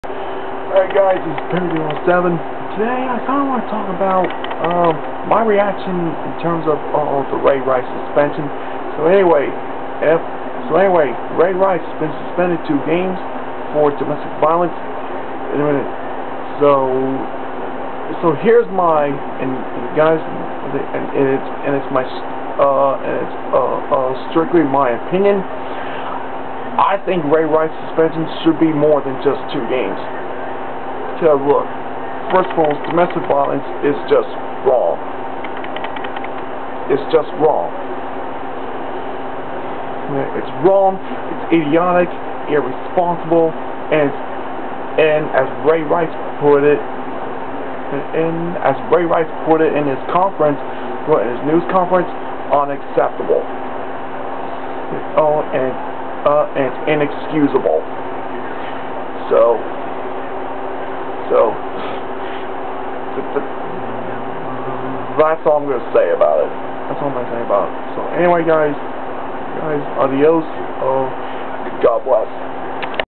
My thoughts on Ray Rice getting suspened 2 games!
Here's my opinion on why Ray Rice should be suspended more than 2 games!